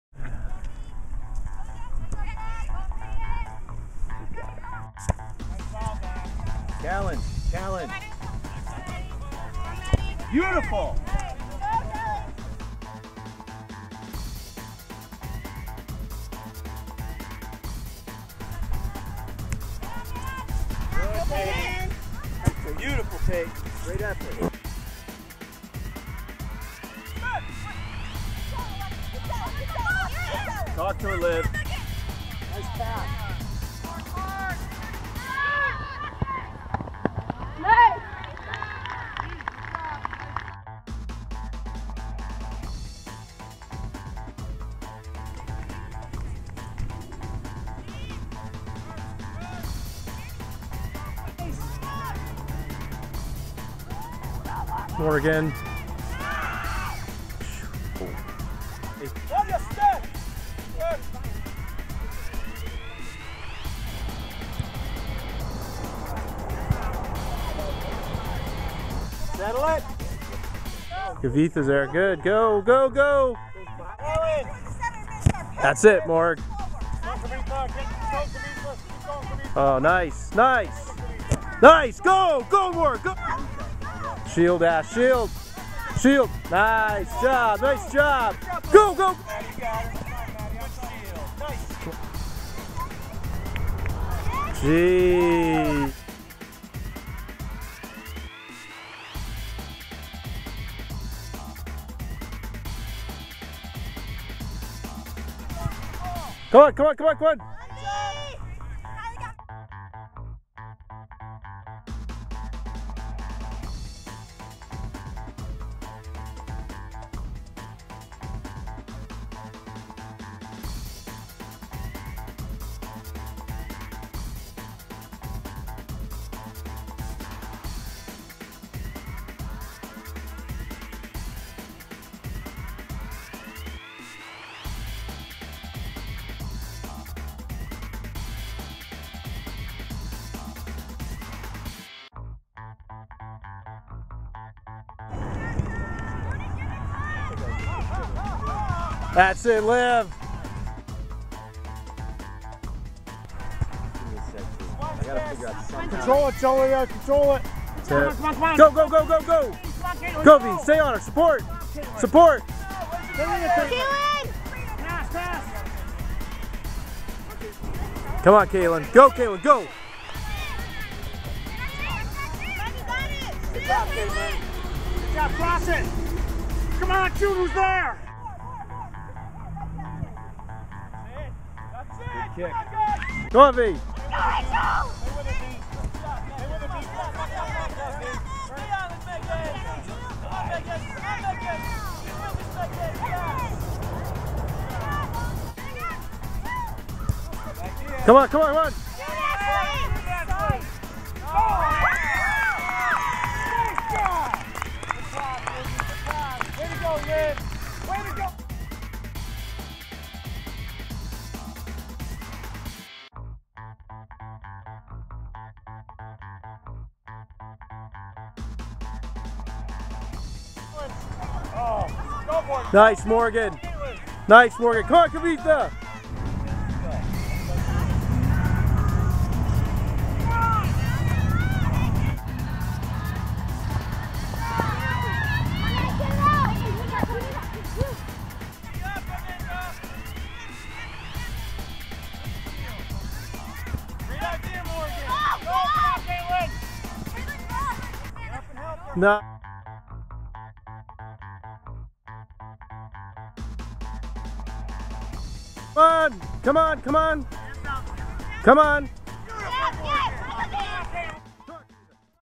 JAmericans vs Newington 11-02-13
great way to end season.  tied game with little time left.  great shielding, trapping and passing.  got unlucky a few times.  fantastic defense.